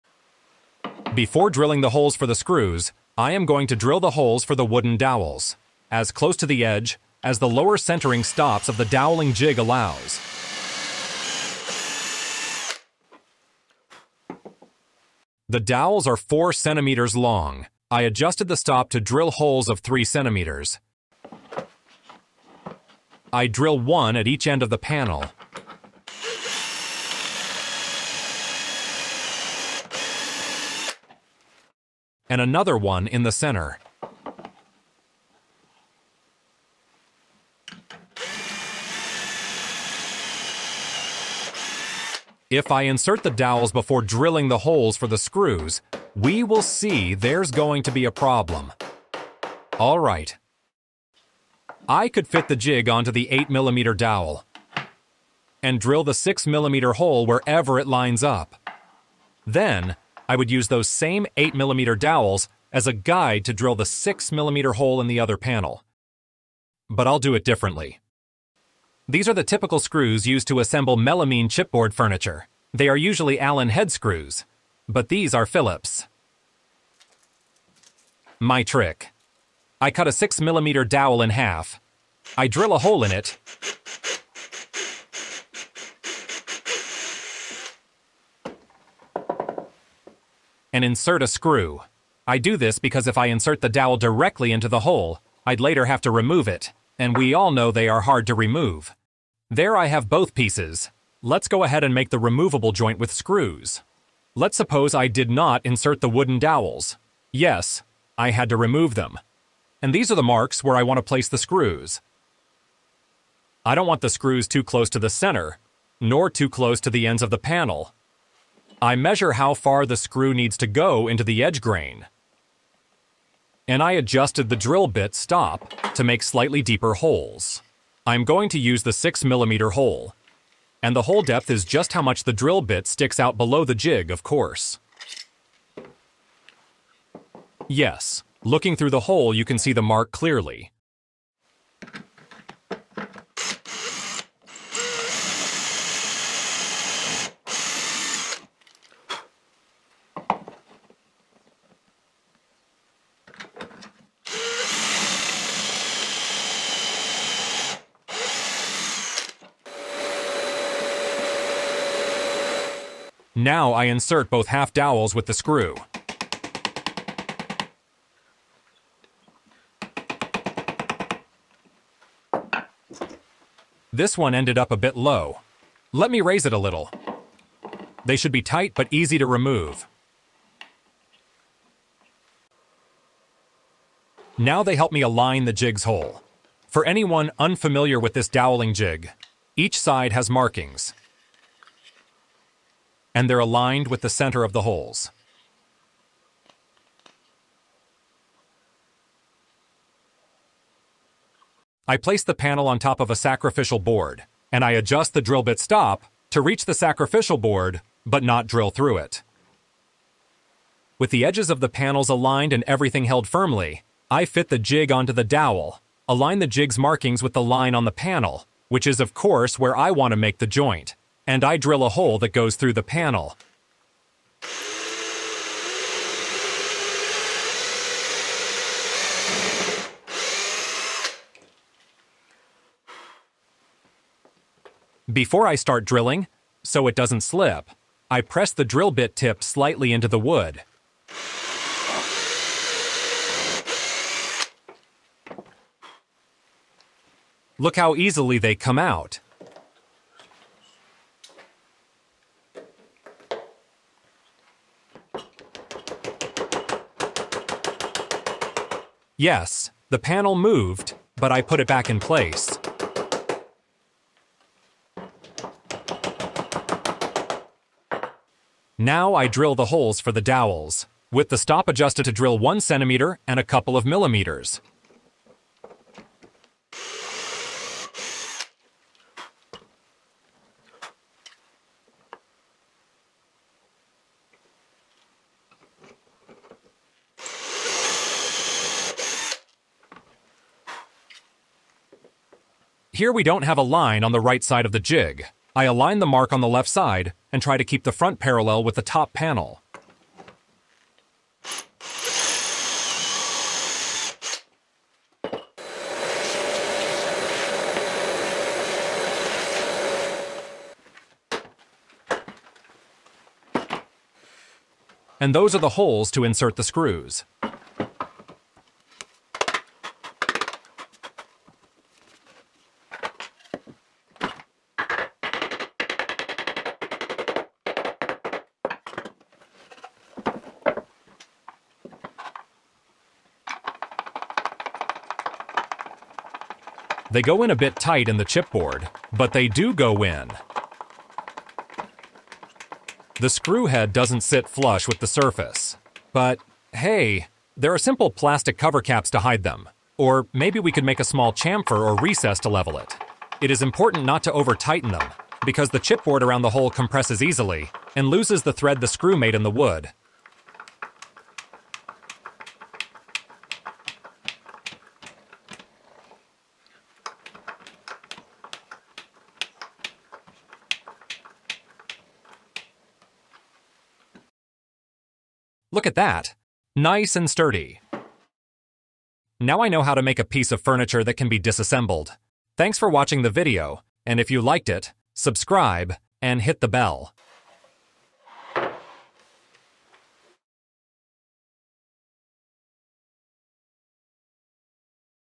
Removable Joints with Confirmat Screws and Dowels in Melamine-Faced Particle Board
This doweling jig is not only useful for dowels joints. We can also use it to make joints with special confirmat screws for melamine-faced particle board furniture. The woodworking trick is to first drill the holes for the dowels and the screws into the edge of the first board. Then, using some half dowels (with a screw inserted to remove them easily), we adjust the position of the doweling jig to drill the through holes for the confirmat screws into the face of the other board. Now we can insert the wooden dowels into the edge of the first board and use them to position the jig and drill the holes for the dowels into the face of the second particle board. The screws I use are the typical ones for Flat-pack furniture made from melamine-faced particle board. A 6-millimeter hole worked for these. Now I know how to join melamine-faced particle boards with dowels and screws, an easy way to make knock-down furniture.

How to use this same doweling jig to build a shelf out of melamine-faced particle board: